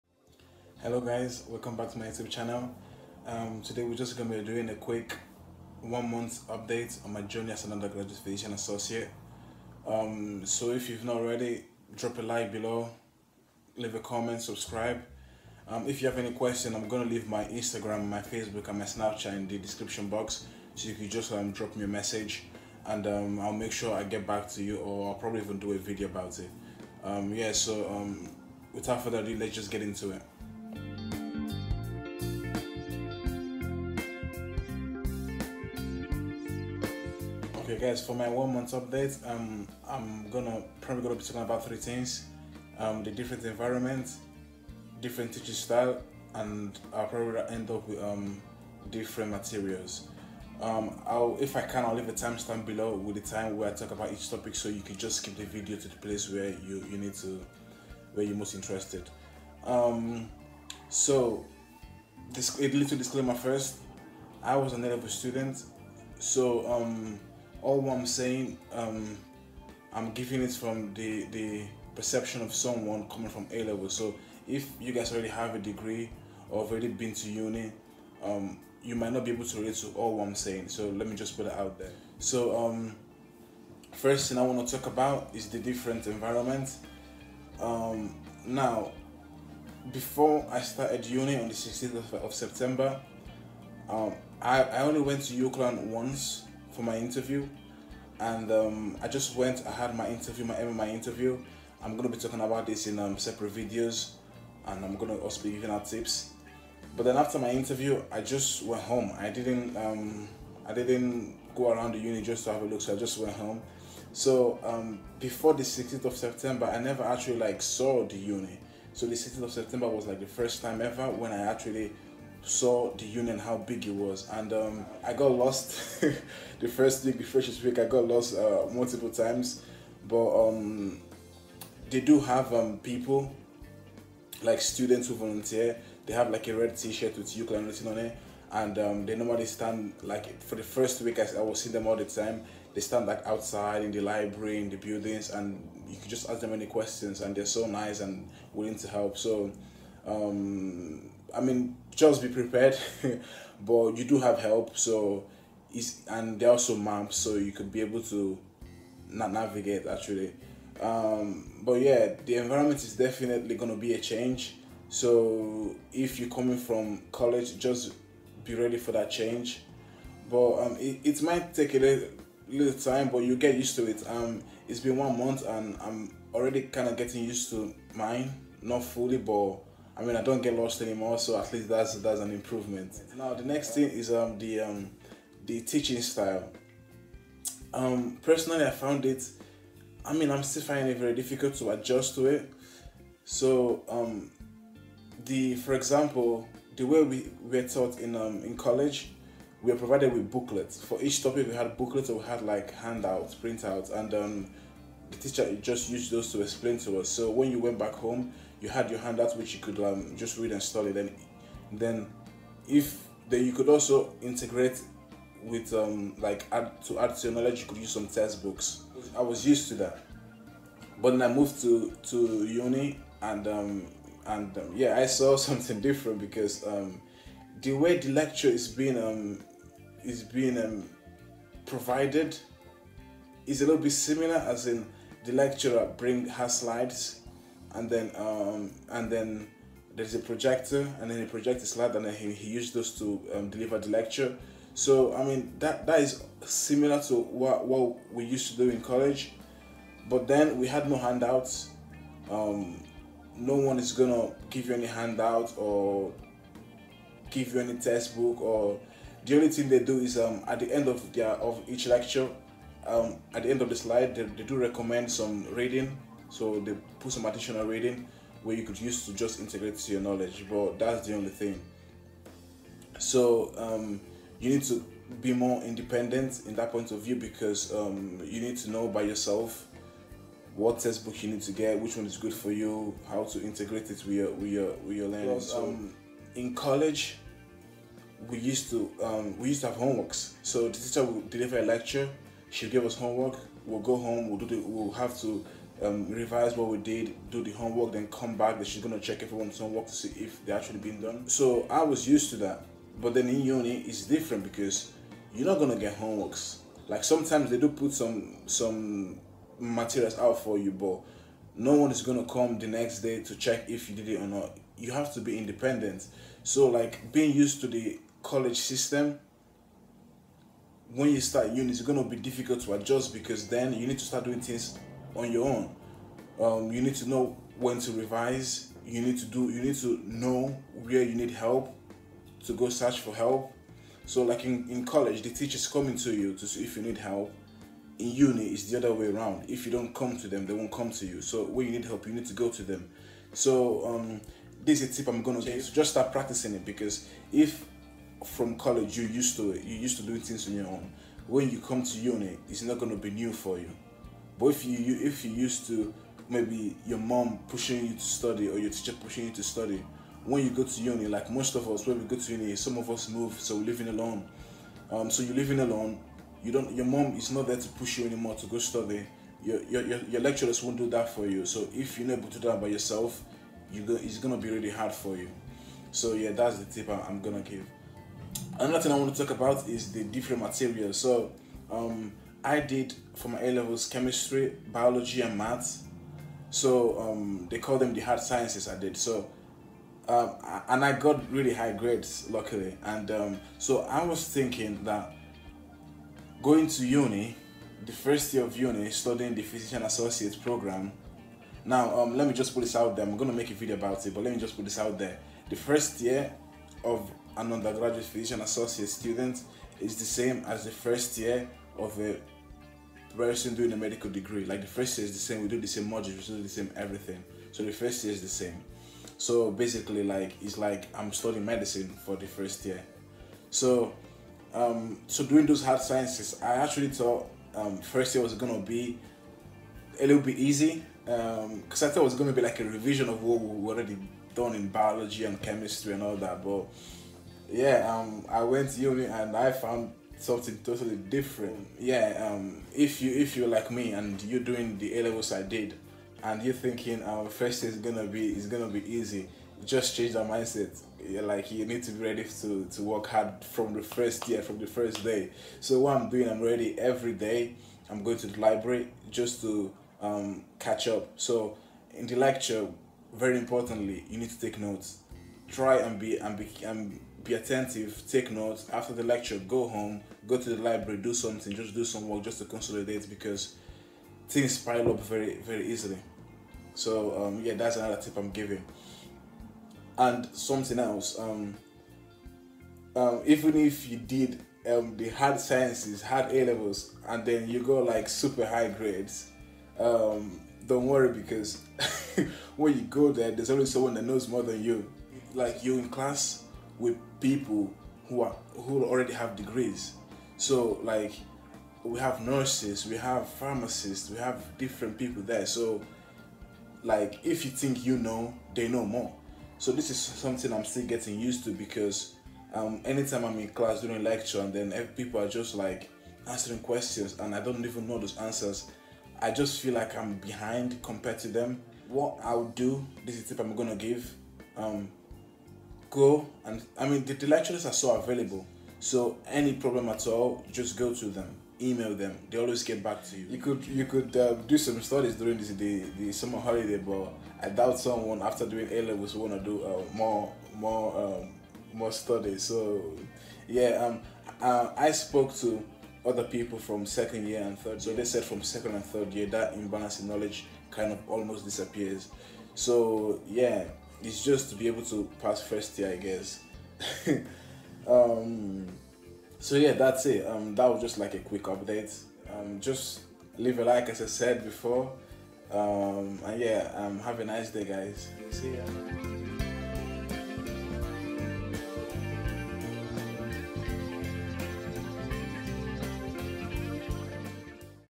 One month update as an undergraduate Physician Associate at UCLAN. (MPAS) UK
Hello guys this is just an update of my journey. These are the topic I covered:
-1:26 The different environment compared to college.
- 3:03 The different teaching style you find in uni and some tips on how to cope and adjust to it
-6:18 The different materials studied compared to the A-levels I did (maths, chemistry and biology)